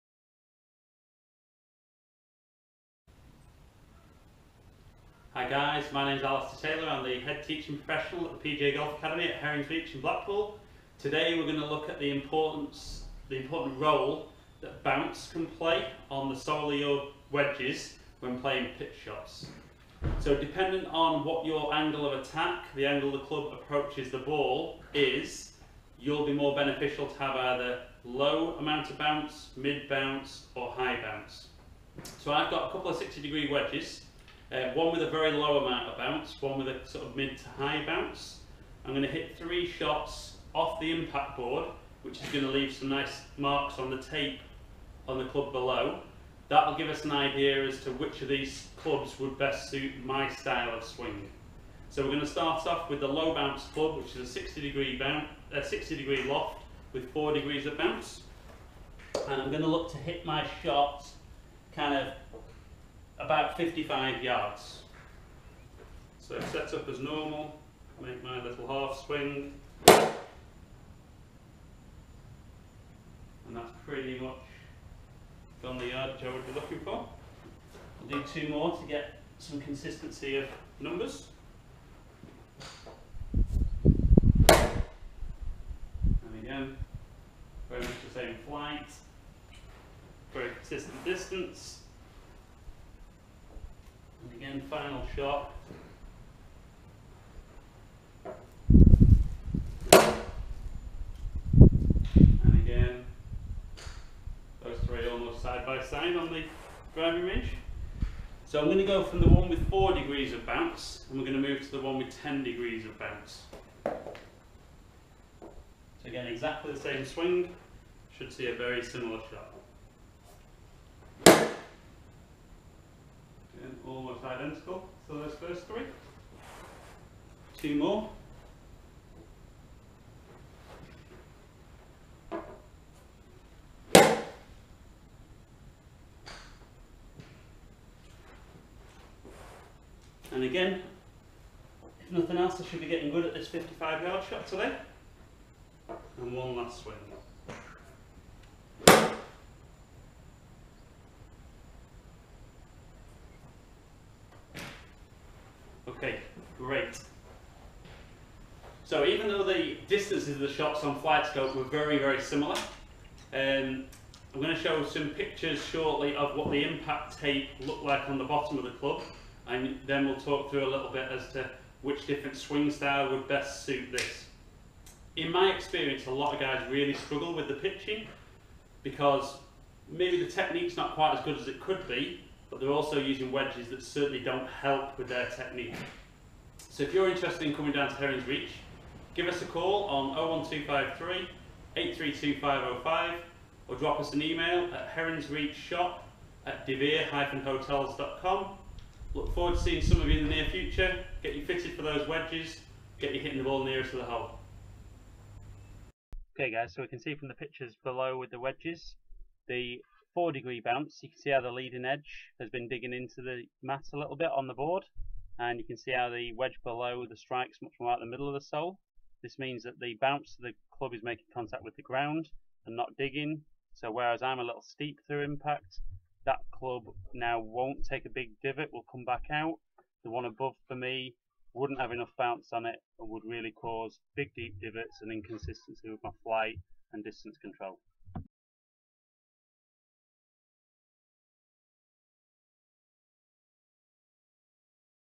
Correct bounce on your wedges
Alastair Taylor runs through the importance of having the correct amount of bounce on your wedges and how this can transform your pitching.